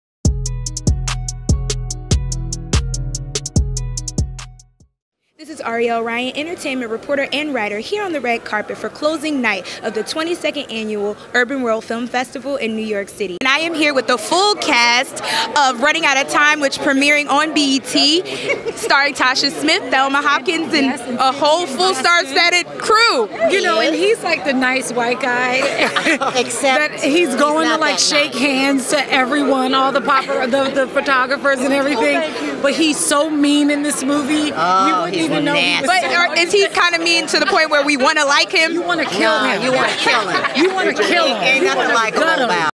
TashaSmith & Cast of BET's "Running Out of Time x
Arielle Ryan chats with Tasha Smith about her latest project released on BET "Running Out of Time." She discusses DustinHarnish and his role as the "Bad Guy."